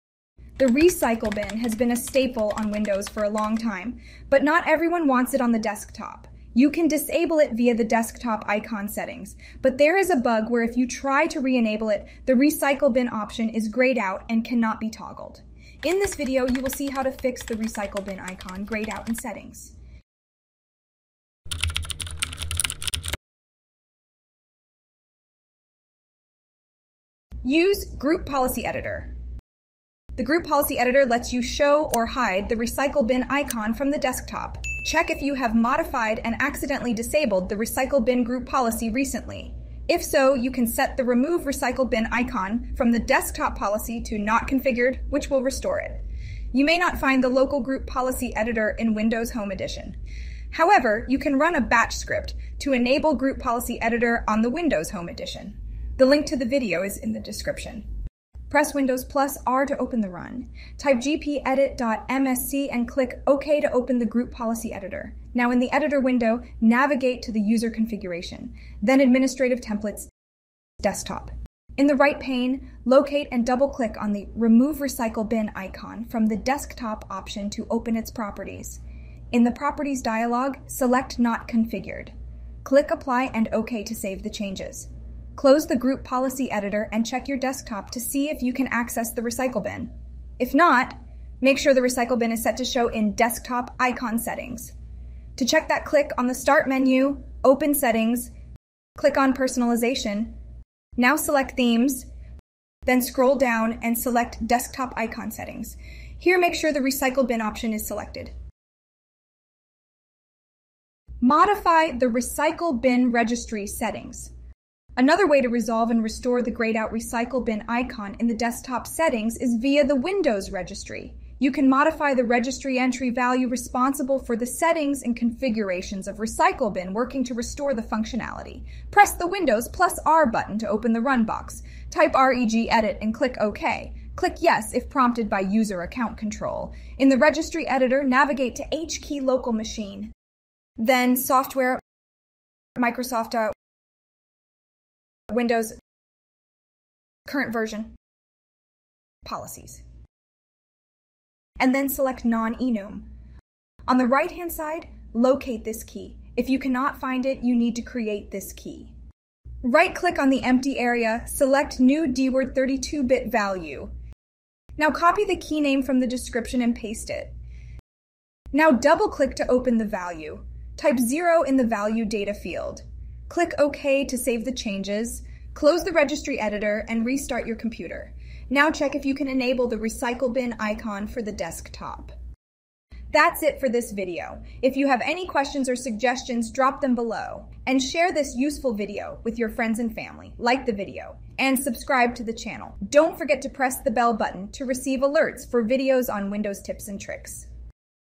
How to Fix the Recycle Bin Icon Setting Grayed Out in Windows 11?
How to fix the recycle bin settings grayed out in Windows 11? The Recycle Bin has been a staple on Windows for a long time, but not everyone wants it on the desktop. You can disable it via the Desktop Icon Settings, but there is a bug where if you try to re-enable it, the "Recycle Bin" option is grayed out and cannot be toggled. In this video, you will see how to fix the Recycle Bin Icon grayed out in settings.

0:00 Intro
0:27 Use Group Policy Editor
1:51 Modify the Recycle Bin Registry Settings